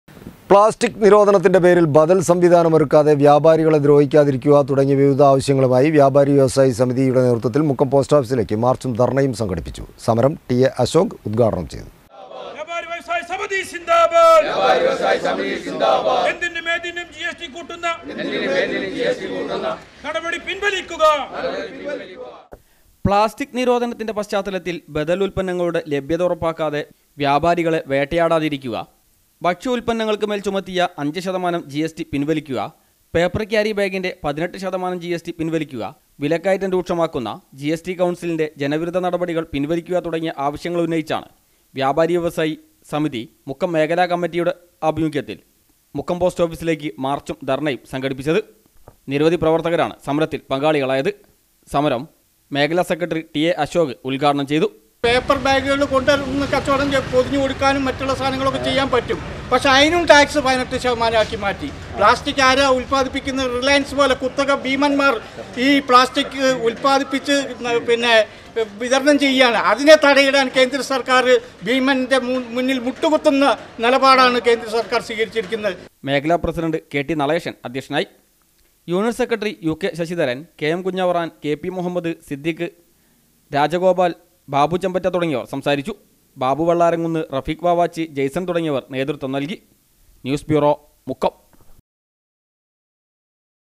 വ്യാപാരി വ്യവസായി സമിതി നേതൃത്വത്തിൽ മുക്കം പോസ്റ്റാേഫീസിലേക്ക് മാർച്ചും ധർണ്ണയും നടത്തി
പ്ലാസ്റ്റിക് നിരോധനത്തിൻ്റെ പേരിൽ ബദൽ സംവിധാനമൊരുക്കാതെ വ്യാപാരി ദ്രോഹിക്കാതിരിക്കുക തുടങ്ങി വിവിധ ആവശ്യങ്ങളുമായി വ്യാപാരി വ്യവസായി സമിതി നേതൃത്വത്തിൽ മുക്കം പോസ്റ്റാേഫീസിലേക്ക് മാർച്ചും ധർണ്ണയും നടത്തി. സമരം ടി.എ അശോക് ഉദ്ഘാടനം ചെയ്തു